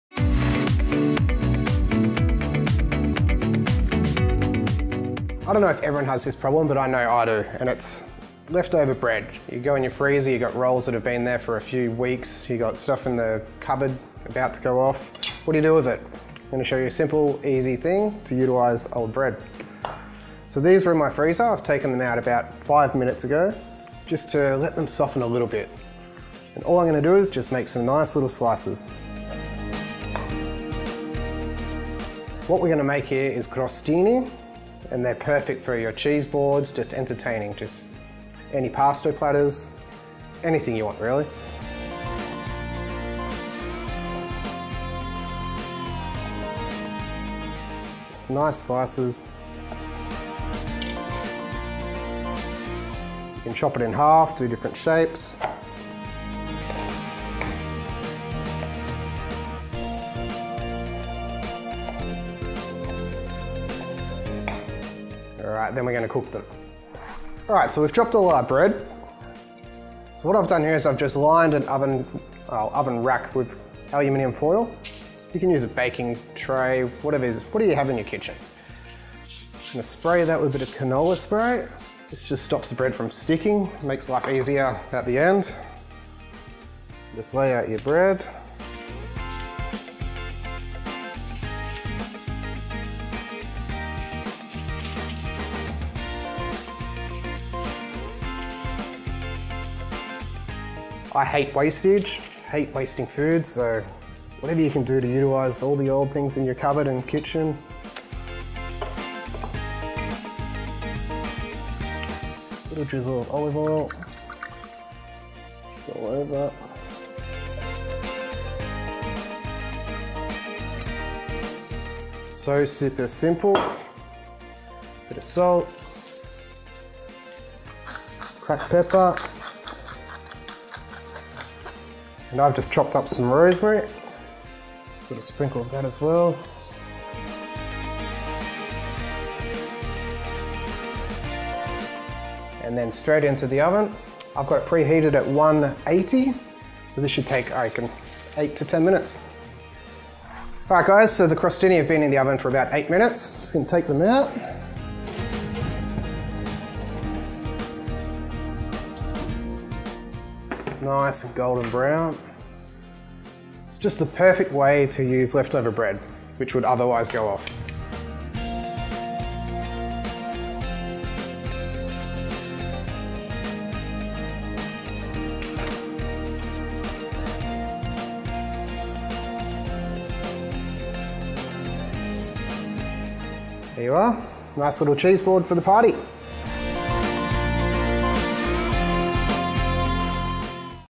Making Crostini
Momento Hospitality teaches you how to make the perfect Crostini.